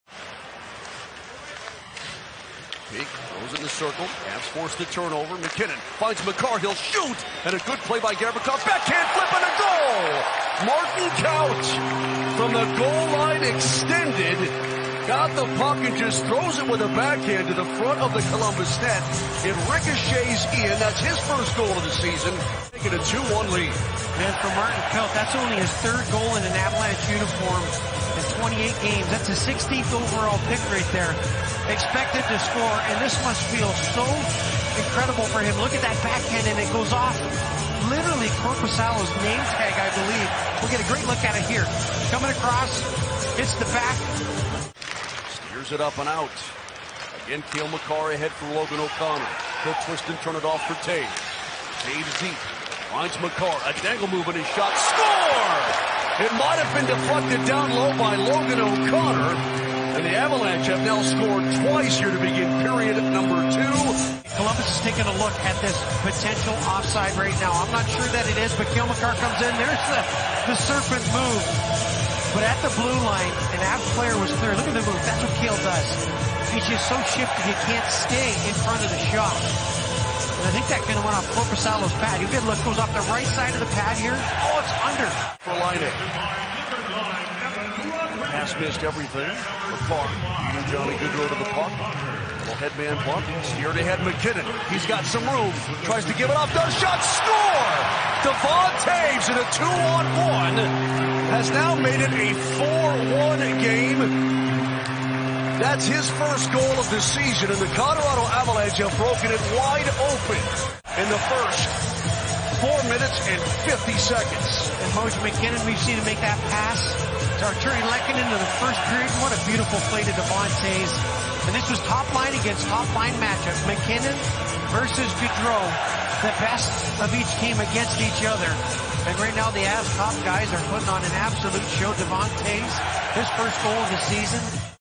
The Avalanche are on fire in the 2nd!
Martin Kaut, Logan O'Connor and Devon Toews each score in the opening 5 minutes of the 2nd period, giving the Avalanche a 4-1 lead over the Blue Jackets in the NHL Global Series in Finland